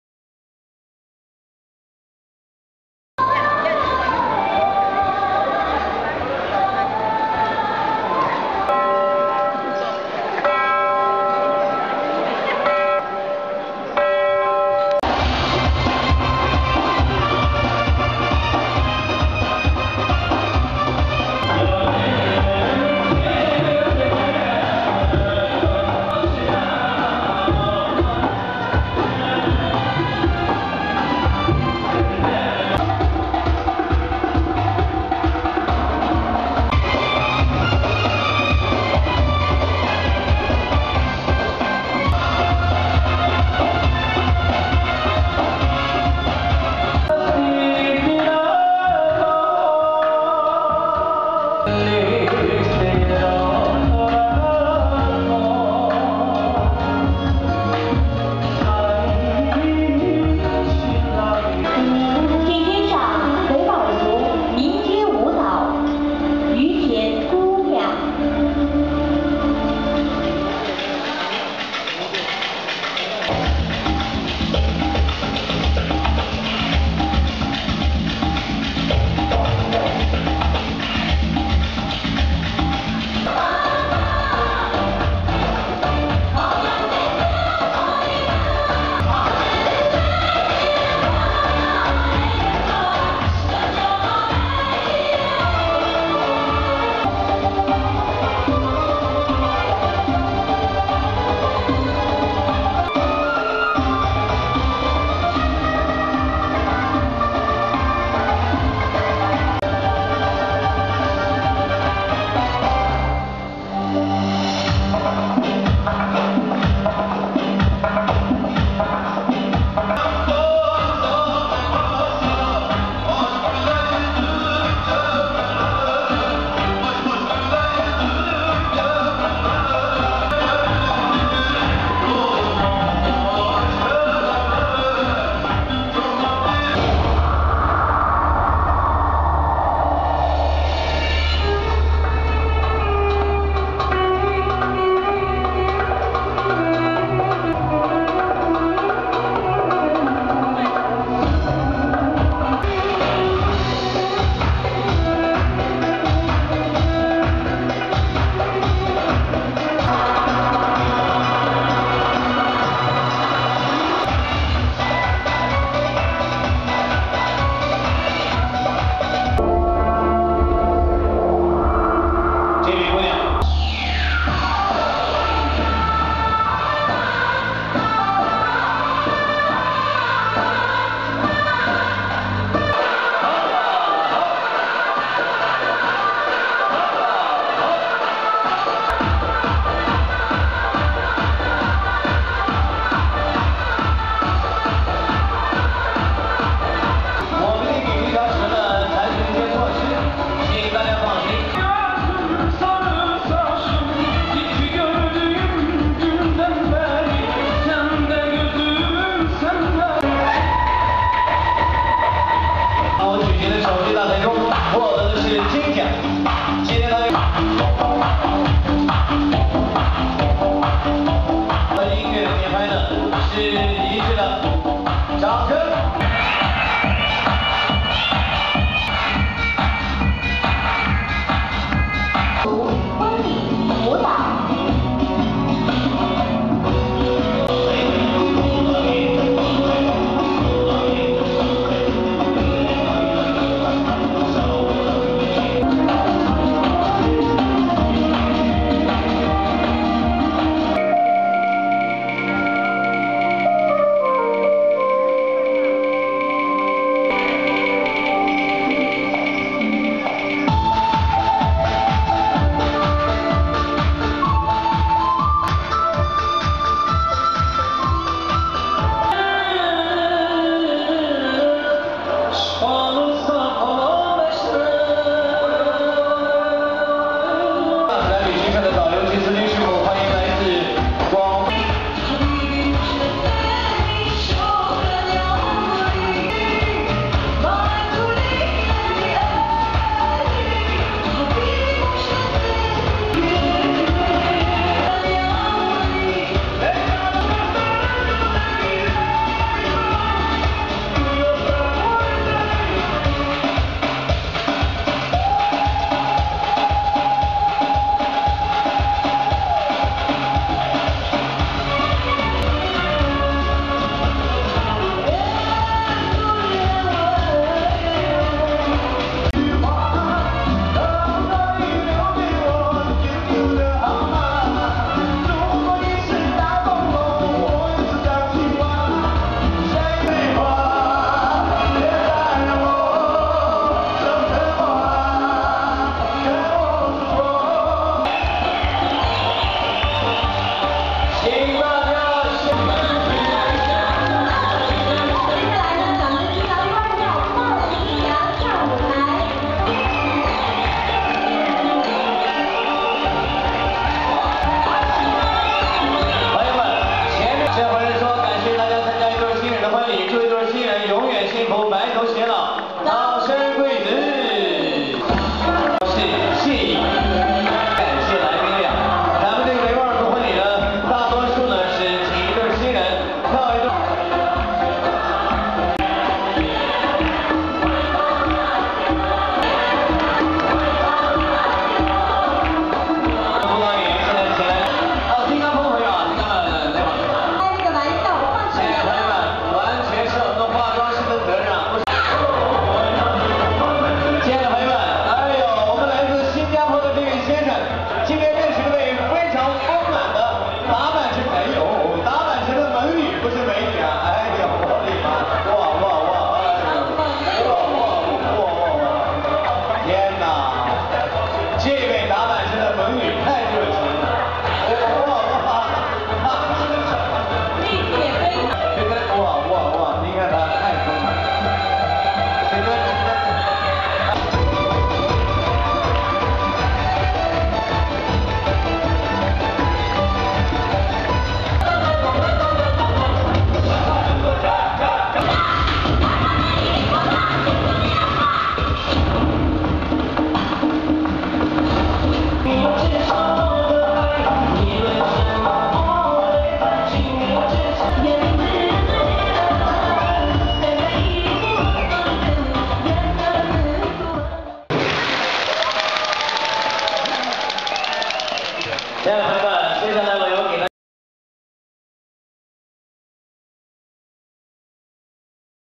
Silk Road - Song and Dance.mpg
This video records a sample of the colourful and vibrant song and dance by the Xinjiang minorities